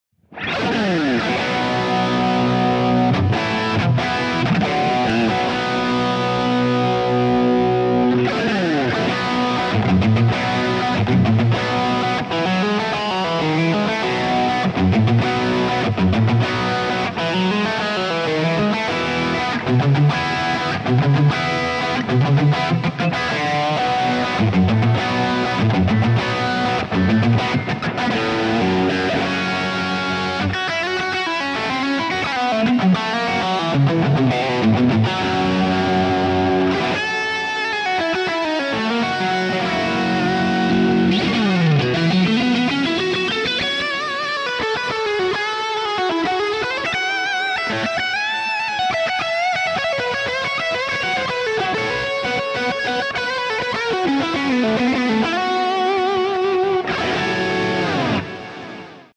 Fender Stratocaster Rock Guitar Jam | by Dave Devlin
What's up, Guys! Welcome to another quick Guitar Jam video. For this I'm using my American standard Fender Stratocaster, with a Seymour Duncan Hot Rails Installed In the bridge position for some thick rock 'n roll tones!! I hope you enjoy the video!
Dave.